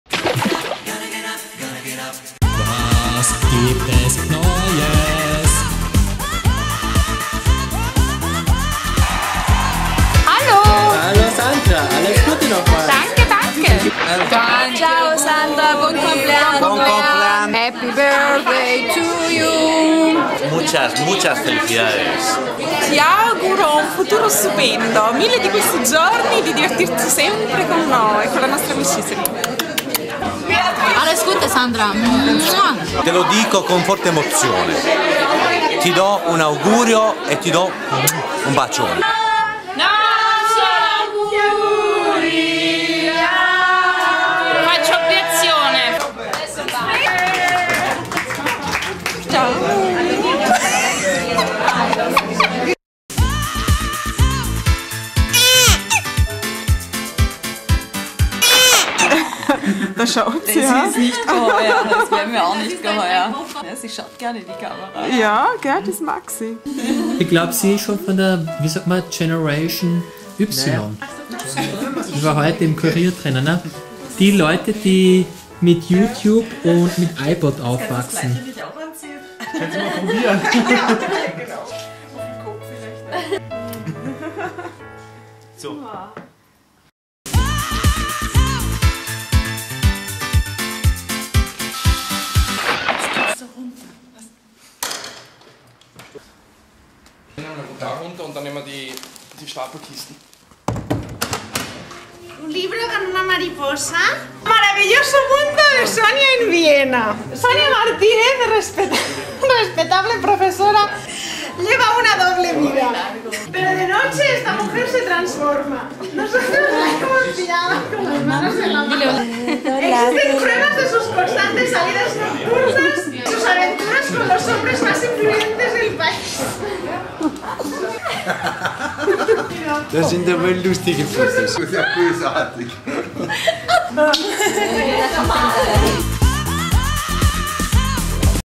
📰 Was gibt es Neues? News from Vienna (Year 2008)
Was gibt es Neues in der Wiener Community?
1) Happy Birthday Party Sandra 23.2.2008
2) New Star on YouTube Sofia 6.4.2008
3) Adios Sonia & Christian 9.2.2008

❤ I like Mani